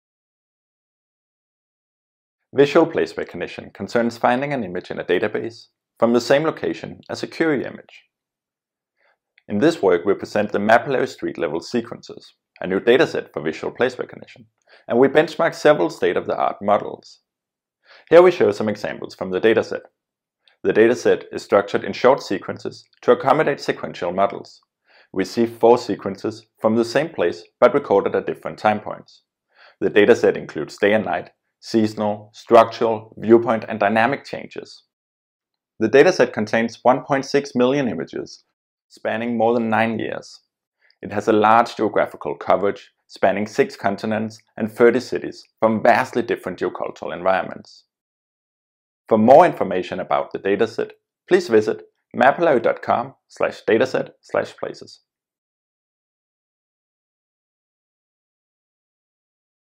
Mapillary Street-Level Sequences: A Dataset for Lifelong Place Recognition - CVPR 2020 (oral)
1-minute spotlight presentation for the CVPR 2020 paper:

Frederik Warburg, Søren Hauberg, Manuel Lopez-Antequera, Pau Gargallo,
Yubin Kuang, Javier Civera
Mapillary Street-Level Sequences: A Dataset for Lifelong Place Recognition
CVPR 2020 (oral)

Abstract:
Lifelong place recognition is an essential and challenging task in computer vision, with vast applications in robust localization and efficient large-scale 3D reconstruction. Progress is currently hindered by a lack of large, diverse, publicly available datasets. We contribute with Mapillary Street-Level Sequences (MSLS), a large dataset for urban and suburban place recognition from image sequences. It contains more than 1.6 million images curated from the Mapillary collaborative mapping platform. The dataset is orders of magnitude larger than current
data sources, and is designed to reflect the diversities of true lifelong learning. It features images from 30 major cities across six continents, hundreds of distinct cameras, and substantially different viewpoints and capture times, spanning all seasons over a nine-year period. All images are geo-located with GPS and compass, and feature high-level attributes such as road type.
We propose a set of benchmark tasks designed to push state-of-the-art performance and provide baseline studies. We show that current state-of-the-art methods still have a long way to go, and that the lack of diversity in existing datasets has prevented generalization to new environments. The dataset and benchmarks are available for academic research.